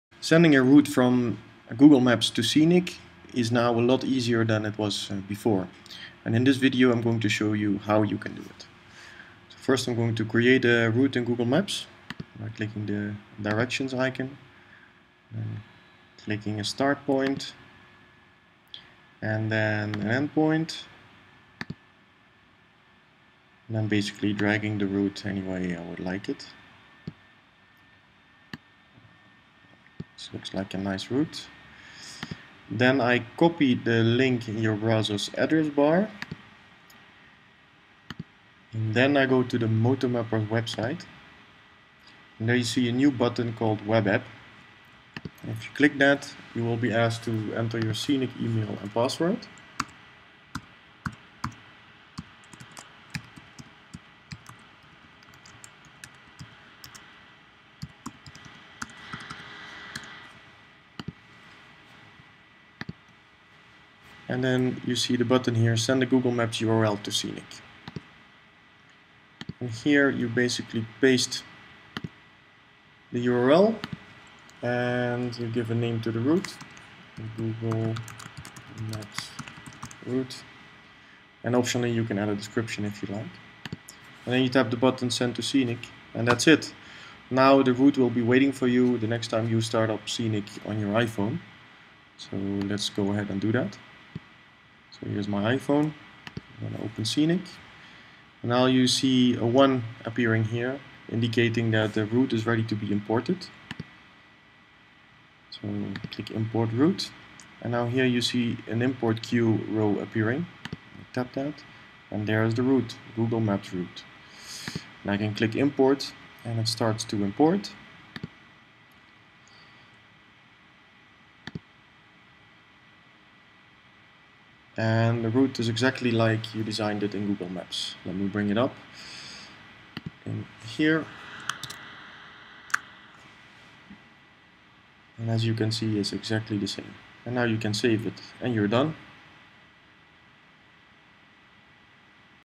New and Easy way to send Google Maps Routes to Scenic
Sending a Google Maps route to Scenic is now easier than ever. Watch this video to show you how.